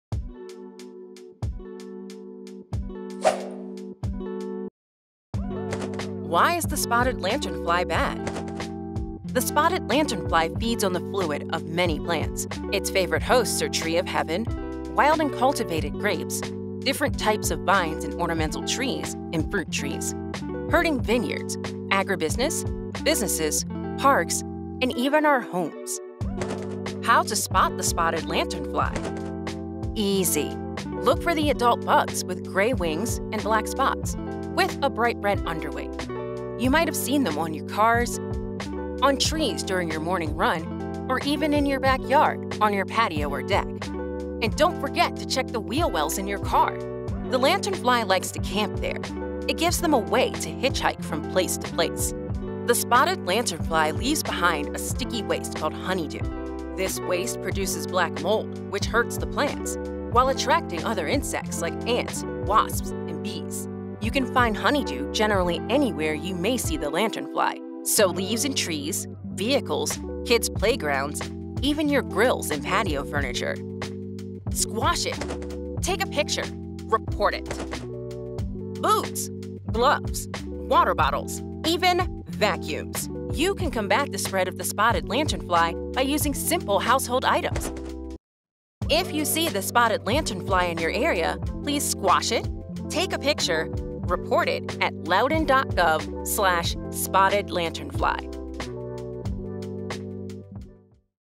Stop The Spread of the Spotted Lanternfly
The invasive and destructive spotted lanternfly has been found in Loudoun County, signaling a new stage in efforts to limit the damage the insect can cause to agricultural crops and home gardens. Learn about the spotted lanternfly, how to recognize the insect throughout its life cycle, and what to do if you encounter one.